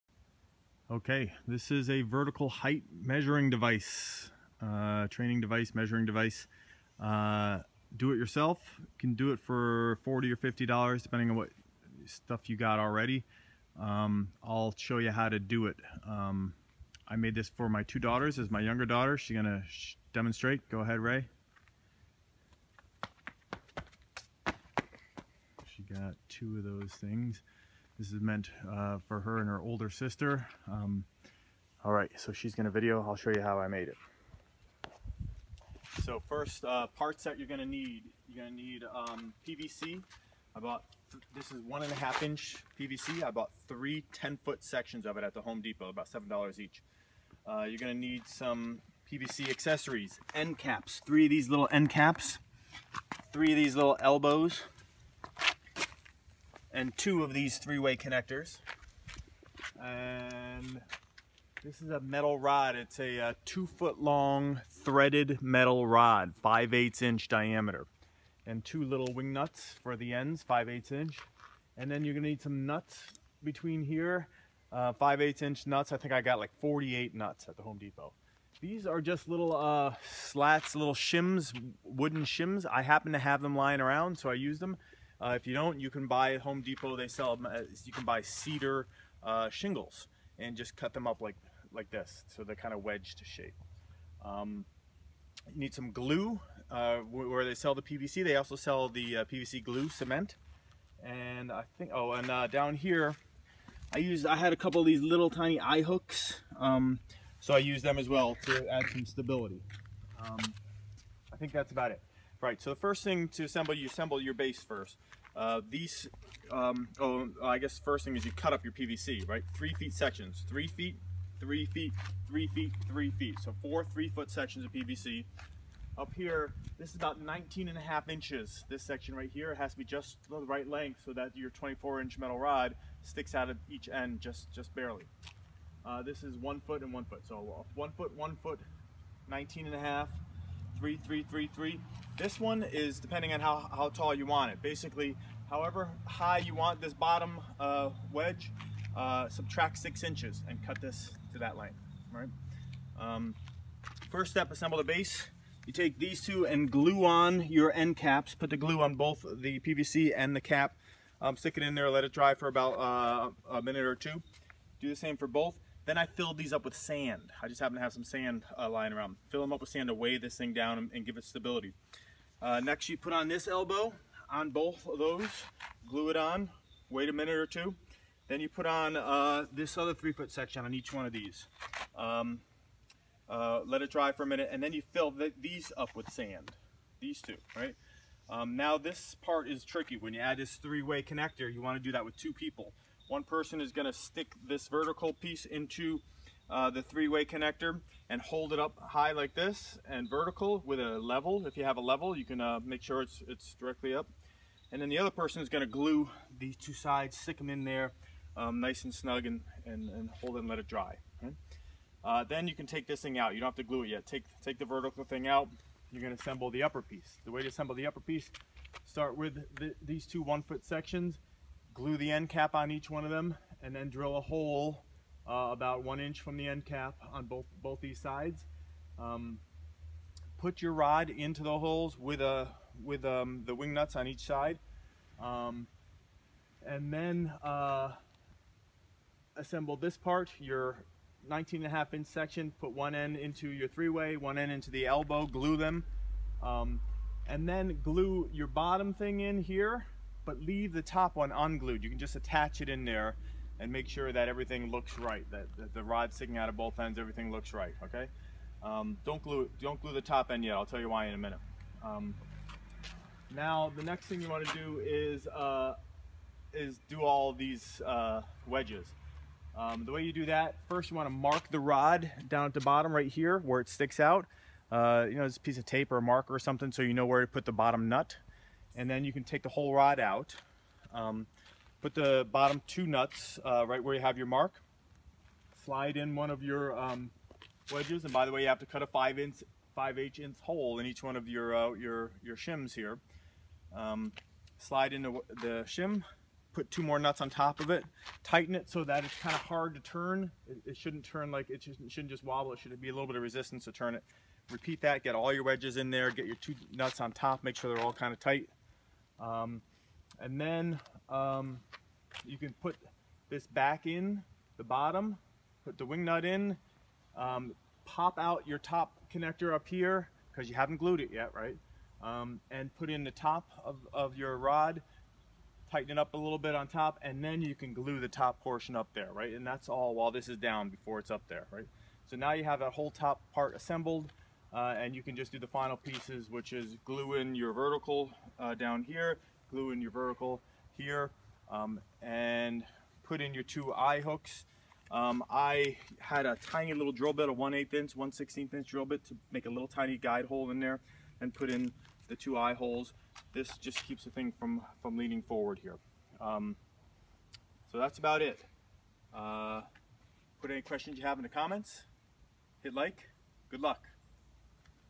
DIY Vertical Leap measuring device
I built this for my 2 daughters - both are into track and field and I couldn't figure out how to build a cheap high jump pit so this was the next best thing.  These things sell for around $350.  You should be able to buy the materals you need at home depot for under $50.

Tools: drill with 5/16" bit and 1/16" bit
saw to cut PVC  - table saw or circular saw or hand saw

Materials:
3x10' 1.5" PVC pipe
3 elbows
two 3-way connectors
3 end-caps
PVC glue/cement
24" threaded metal rod 5/16"
48 nuts 5/16"
24 wood shims about 1" x 10" each (you can make these out of cheap cedar shingles from home depot)
some sand (to put into PVC base for weight/stability)
2 tiny eye hooks and some rope

1) Cut PVC to 4 x 3', 1 x 19.5", 2 x 1' and one vertical piece that will be cut somewhere around 7' or 8' depending on how high you want it.  first determine how high you want the lowest shim, then subtract 6 inches and cut to that length.  I was making for my 13yo and 10yo girls - lowest wedge was about 7'10", highest was around 9'6"

2) assemble base - glue end caps onto two 3' sections.  (wait a couple minutes) then add sand to the  3-feet sections with caps - fill sand to top;.  attach elbows to both sides with glue, then attach another 3 foot section to the elbow with glue; fill both sections with sand after attaching; attach 3-way connector - this is tricky and is best done with a partner - you will likely spill a little sand.  put the long vertical section into the middle slot on the 3-way connector - one person hold that vertical (best to use a level) the other person glues the two sand-filled sections into the other ports on the 3-way

3) assemble top part - glue end caps on two 1' sections; drill a 5/16 hole straight through each, about 1" from end-cap.  put rod into these sections and put wing nuts on each end of rod;  glue 2 elbows to each end of the 19.5" section;  glue the bottom 1' section to one elbow, then attach the top 1' section to the other elbow but don't glue yet.

4) add your shims: drill a 5/16" hole in the back side of each shim (careful you don't break the shim);  while rod is still in the pvc with two wing nuts on, mark the bottom of the rod exposed above the bottom PVC so you know where to put the bottom nut;  remove rod from PVC; put two nuts on the rod - bottom one at your mark, top nut right against the bottom nut - put a shim on, put two more nuts on and tighten until shim is a little hard to move around;  repeat this process until all shims are on and put two nuts on top;

5) Finish 'er up: put bottom of rod into pvc and put wing nut barely on bottom - detach the top pvc from the elbow (this is why you didn't glue it!) and put the top of rod through holes in PVC, then glue back onto the elbow;  gluethe top apparatus to the vertical pole;  glue it all onto the base. drill two tiny holes into the end caps of the base facing up (smallest bit you have maybe 1/16 or 1/8); screw in two small eye hooks;  attach rope as shown